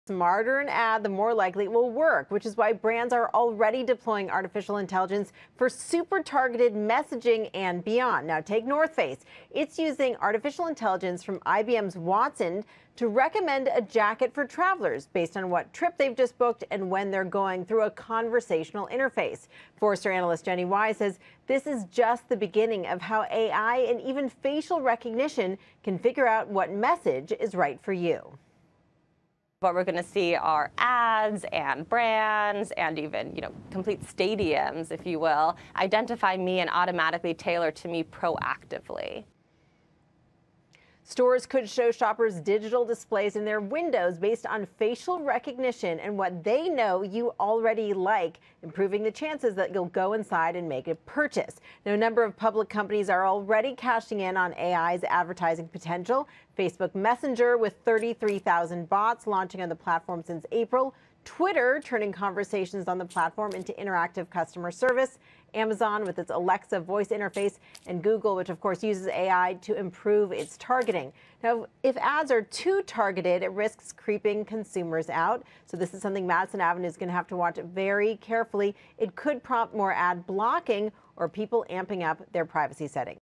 Artificial Intelligence (AI) in advertising
While for some the notion of AI represents a step into science fiction (or at least science future), there are iterations that have real world implications at this moment. This version of AI will probably not bring about downfall of humanity, but rather be used to shape how advertising is created and targeted.”